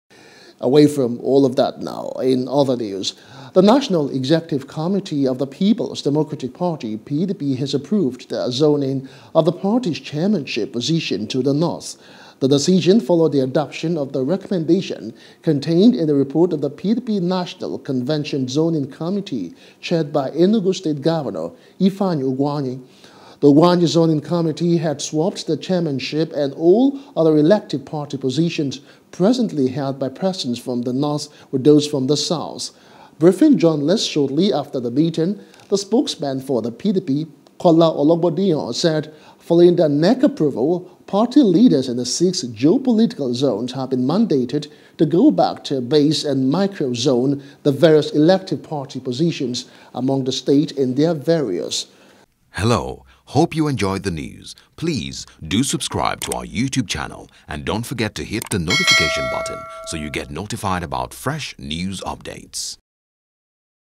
P.D.P, N.E.C Approves Zoning Of Chairman To North | NEWS
The National Executive Committee (NEC) of the People's Democratic Party (PDP) has approved the zoning of the party’s chairmanship position to the North.